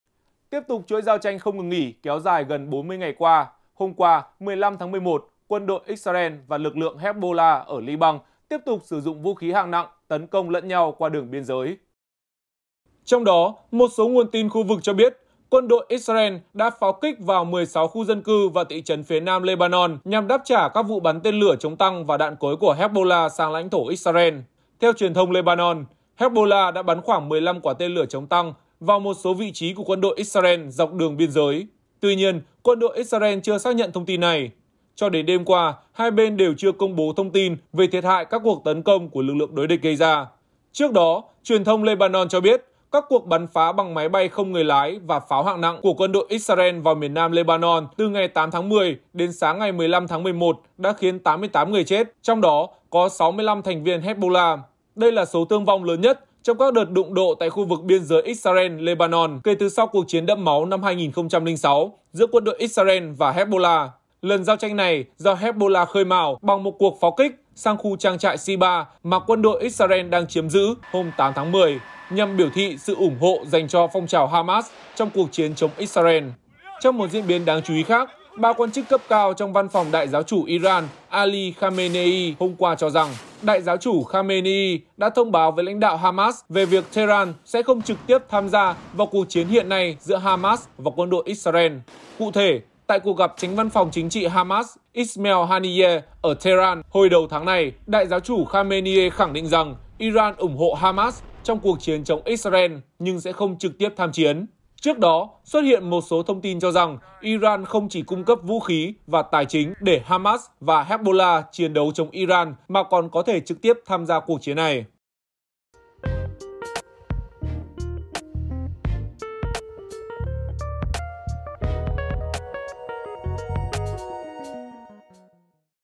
Israel bắn phá 16 thị trấn miền Nam Lebanon, Iran tuyên bố không tham chiến | Báo Điện tử VOV
Báo Điện tử VOV | Tiếp tục chuỗi giao tranh không ngừng nghỉ kéo dài gần 40 ngày qua, hôm qua (15/11), quân đội Israel và lực lượng Hezbollah ở Lebanon tiếp tục sử dụng vũ khí hạng nặng tấn công lẫn nhau qua đường biên giới.

37 Bà Triệu, Hoàn Kiếm, Hà Nội, Việt Nam